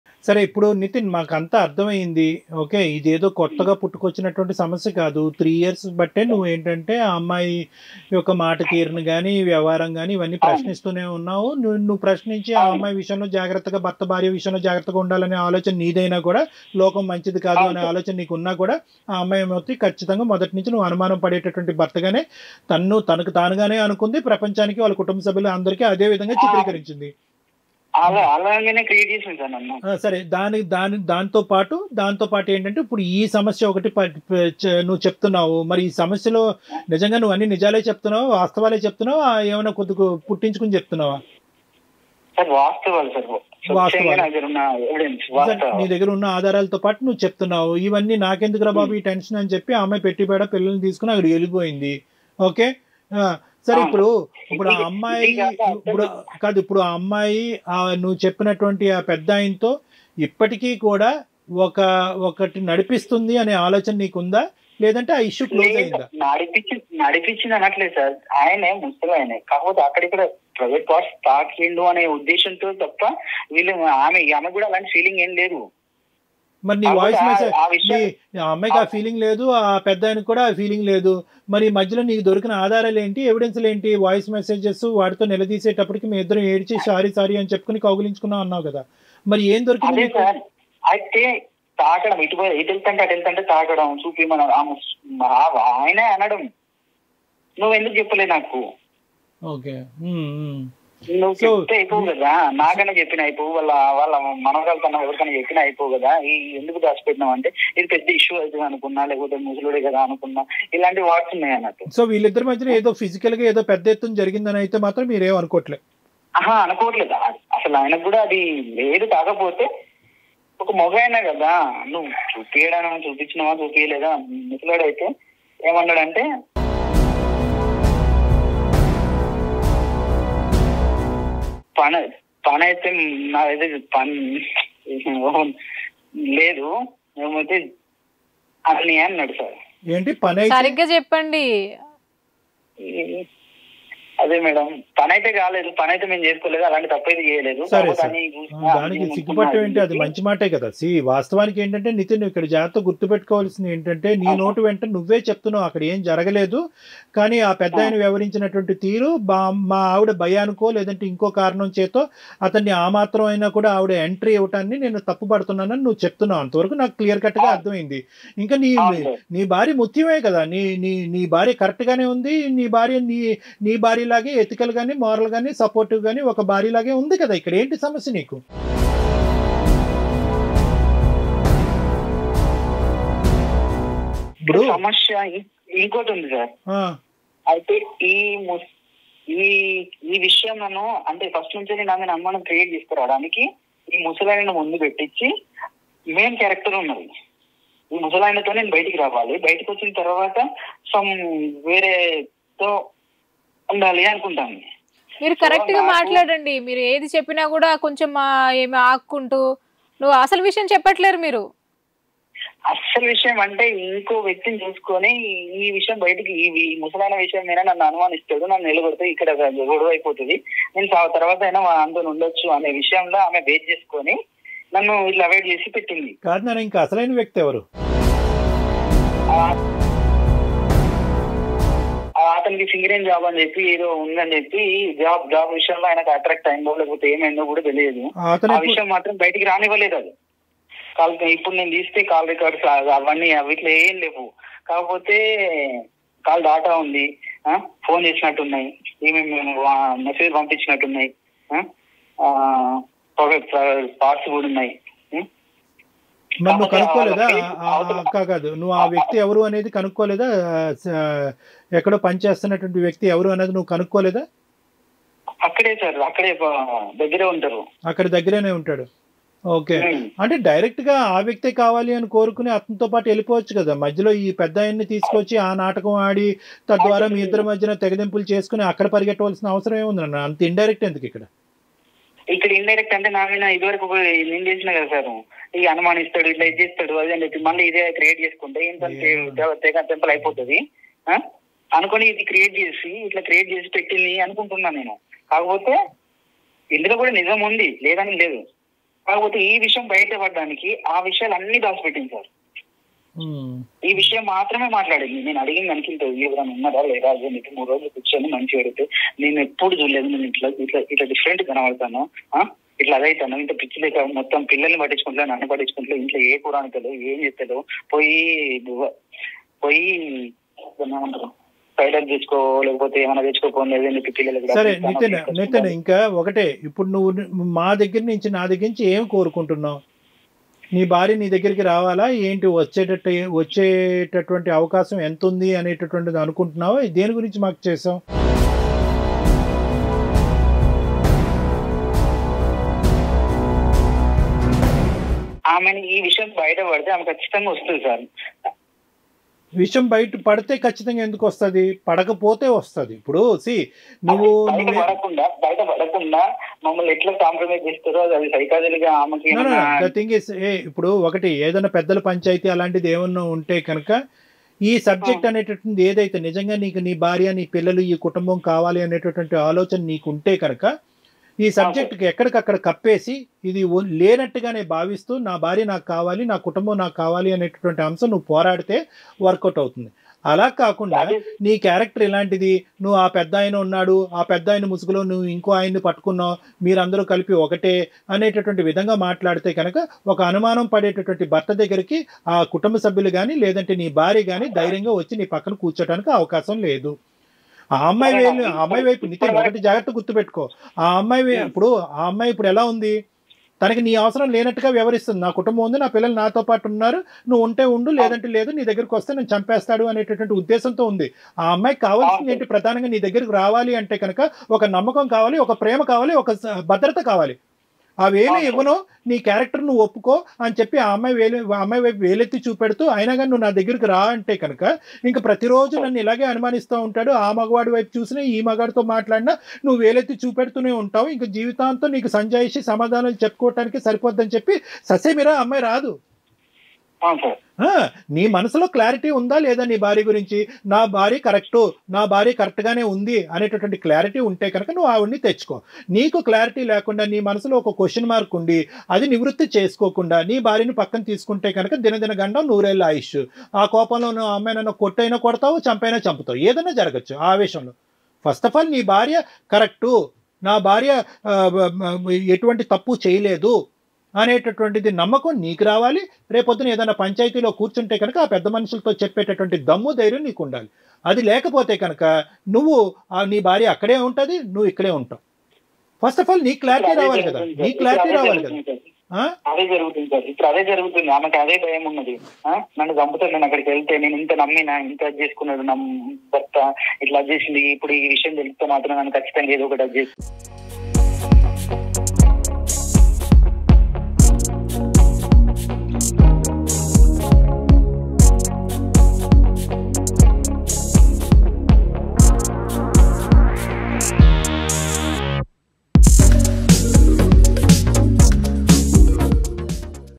మా ఆవిడ ముసలోన్ని అడ్డం పెట్టుకొని వేరే అక్రమసంబంధం.. | Dr.Kalyan Chakravarthy@HitTVExclusive
Watch : మా ఆవిడ ముసలోన్ని అడ్డం పెట్టుకొని వేరే అక్రమసంబంధం.. | Dr.Kalyan Chakravarthy@HitTVExclusive